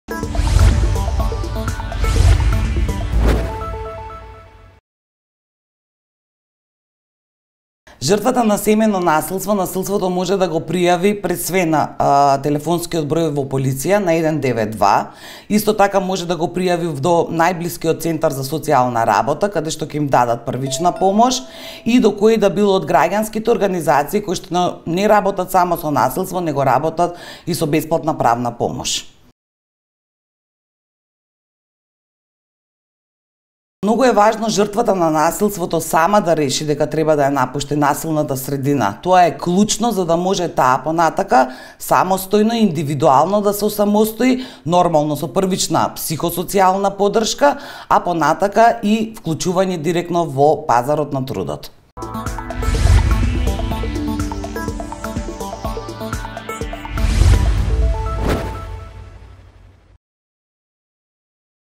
Каде може да се пријави семејно насилство?
Одговара Ана Аврамовска Нушкова од од Националната мрежа против насилство врз жените и семејното насилство